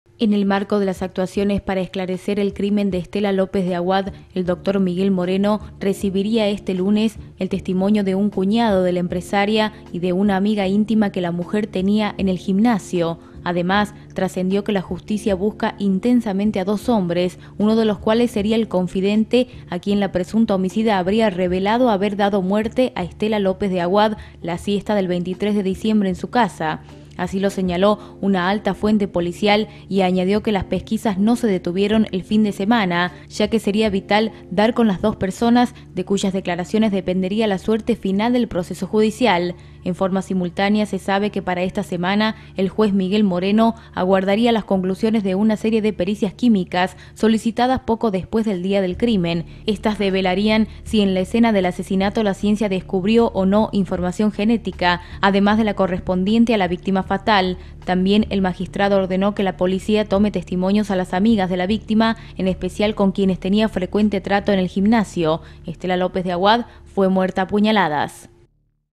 Caso Estela López: el juez tomará nuevos testimonios
El Dr. Miguel Moreno recibiría este lunes el testimonio de un cuñado de la empresaria y de una amiga íntima que la mujer tenía en el gimnasio.